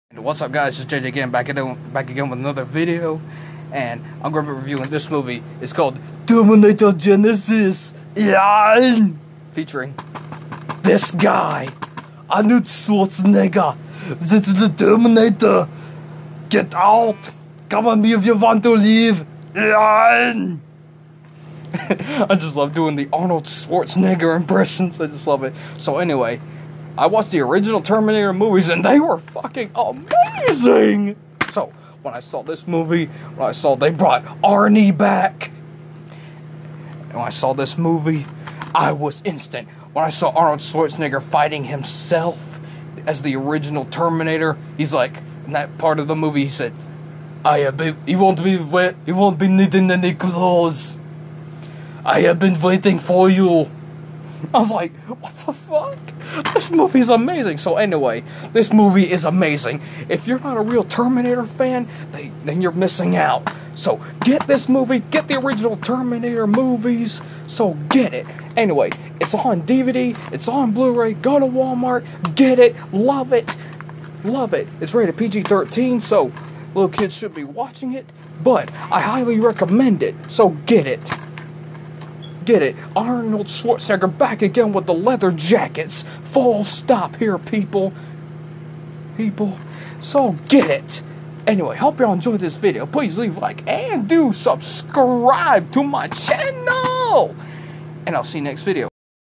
Terminator Genesis movie review
So basically, this is my review on the movie terminator Genesis, I Love this movie, get this movie, get it now, get the original terminator movie series on DVD or blu Ray, get it online, get it, watch it, Love it, so anyway, I'll be on joy, this video please leave like and do subscribe and follow me on amino, and I'll see you next video